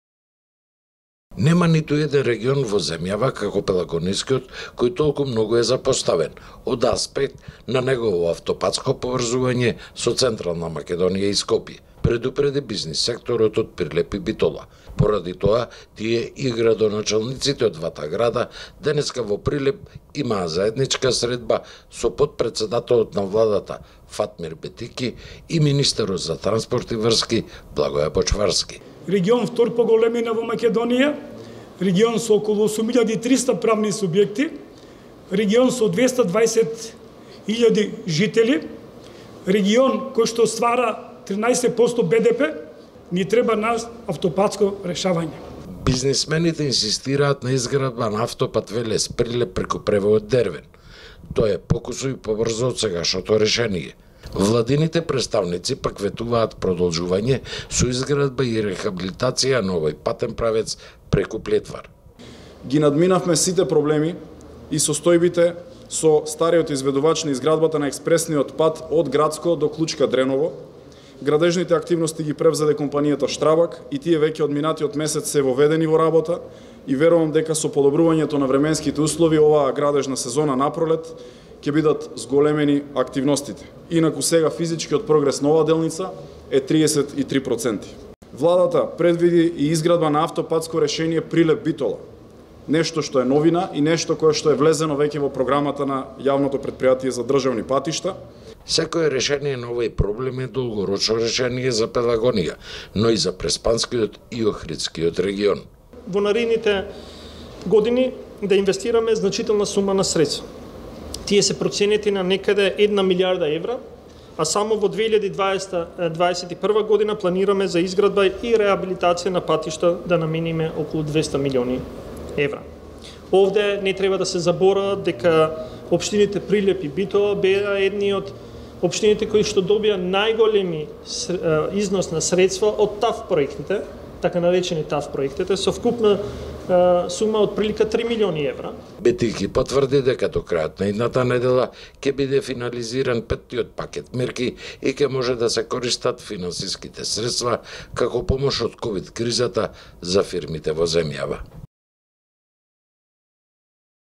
Битиќи и Бочварски во Прилепскиот регион
Вицепремиерот Битиќи и министерот за транспорт и врски Бочварски во Прилеп и прилепско во инспекција на реализацијата на  инфраструктурните проекти.